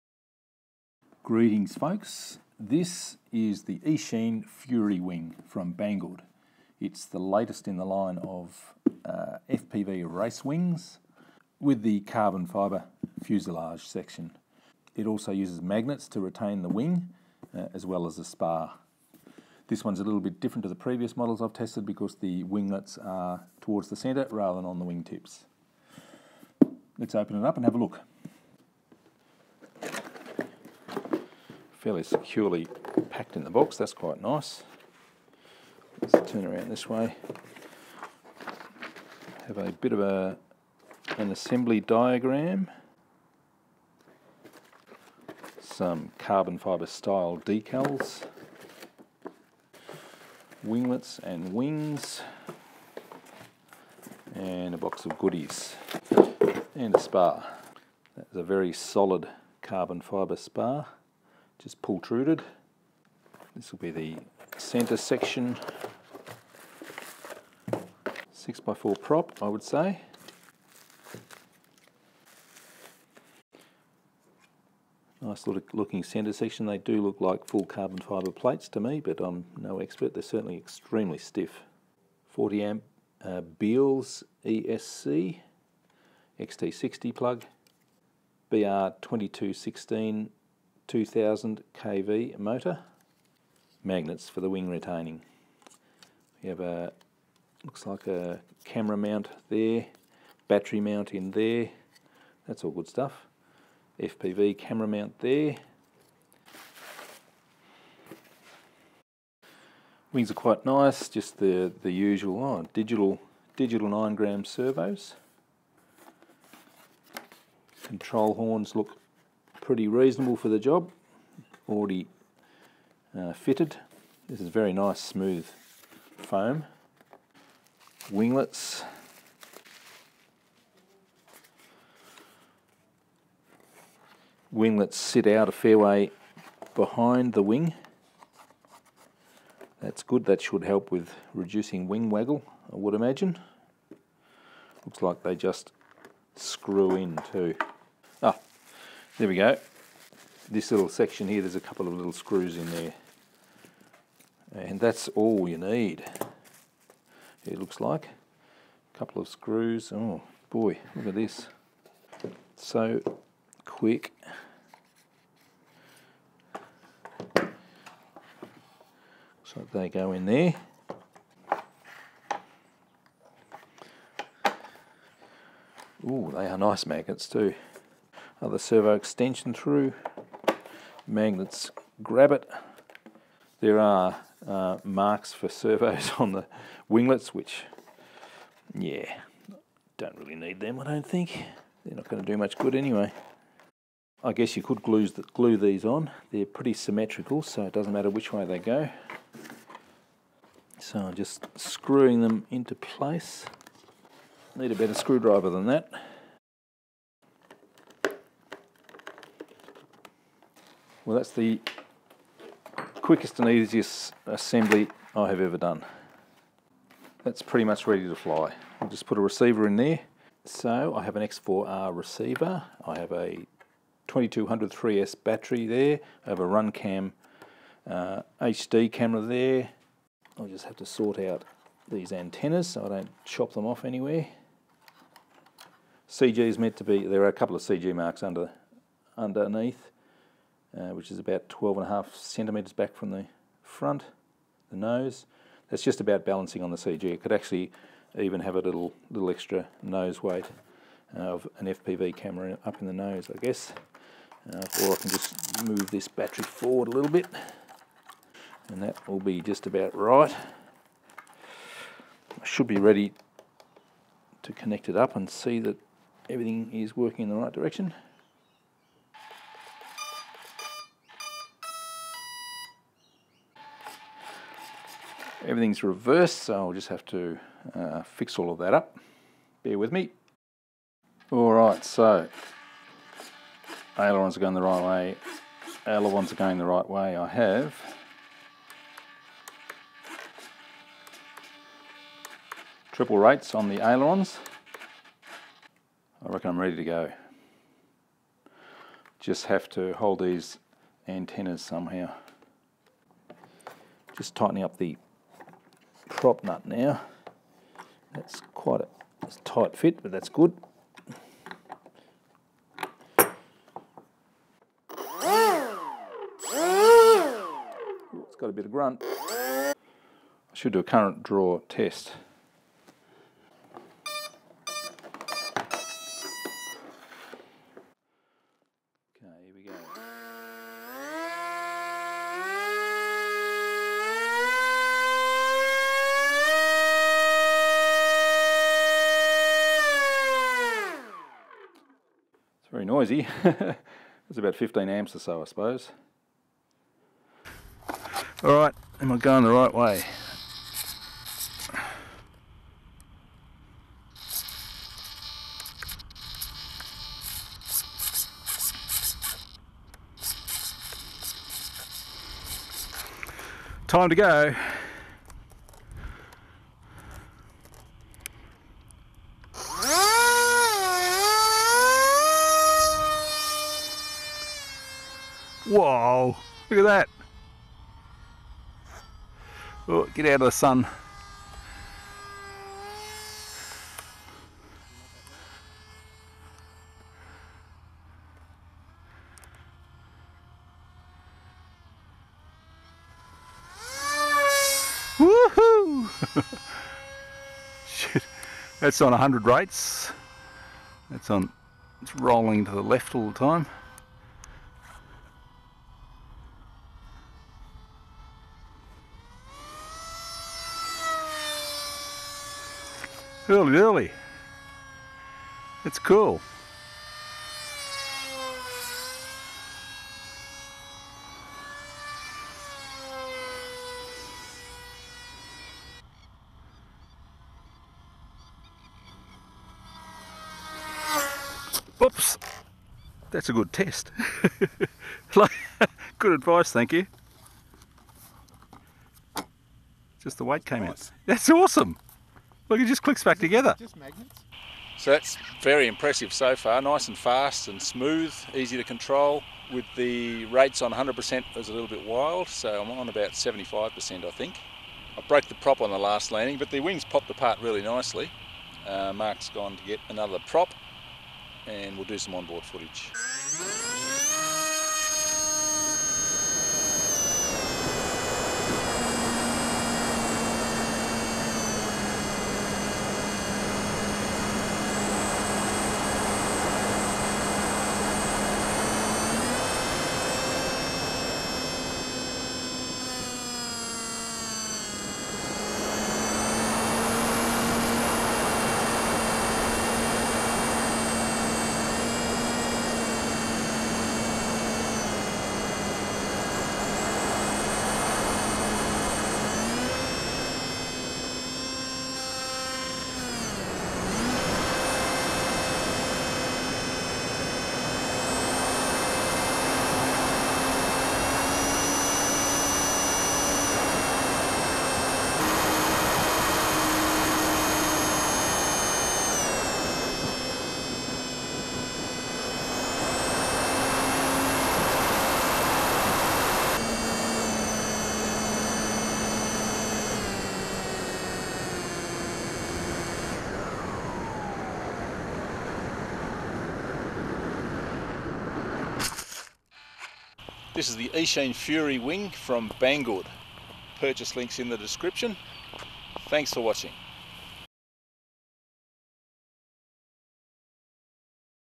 Eachine Fury Wing - Build and maiden
1030mm wingspan FPV racing wing with CF fuselage and EPO wings. Magnet wing retaining system. This is a very nice, fast and smooth wing. Tilt tray for the HD camera.
Description:
Brand Name: Eachine
Item Name: Fury Wing
Wingspan: 1030mm/40.55in
Length: 396mm/15.59in
Color: White
Material: Carbon Fiber Fuselage + EPO Wing
CG: 12.5cm from the wing leading edge(There is a convex point under the wing,that is the CG)
ESC: 40A 3-4s with XT60 connector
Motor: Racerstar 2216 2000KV
Motor Weight: about 66g
Motor Shaft Diameter: 6mm
Propeller: 6x4.5
Servos: 2pcsx 9g digital servo
Flying speed: 20-180+km/h
Compatible cameras: Gopro Session, FOXEER HS1177, Runcam2 HD/Runcam3 HD

Features:
Full carbon fiber fuselage, not Glass Fiber material, this is durable and rigid.
EPO molded wing, pre-built-in Carbon Fiber spar for enhancement, light and flexible plus crash resistence.
Detachable wing and wingtip for easy transport, and easy to change wing by simple plug.
Mainwing connected to fuselage by strong magnets, will be disconnect and release during crash.
Powerful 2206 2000KV Brushless Motor, 40A ESC,servos are all pre-installed for your convenience.
Motor mount can be moved forward or backward to adjust CG.
Multiple camera mount, compatible for FPV camera (FOXEER HS1177) and HD Camera(Gopro Session or Runcam2 HD/Runcam3 HD).
Well protect for your FPV gear and RC components.
With 2 skids for hand launch grip and landing.